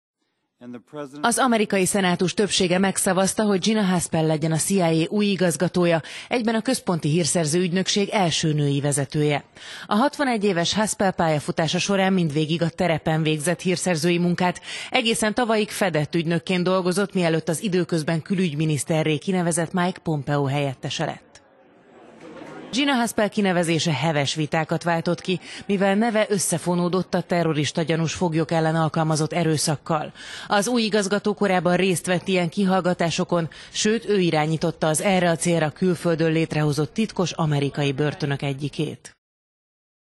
Gina Haspel a CIA első női igazgatója
A terroristagyanús foglyok kínzásában is jártas Haspel lett Mike Pompeo utódja.…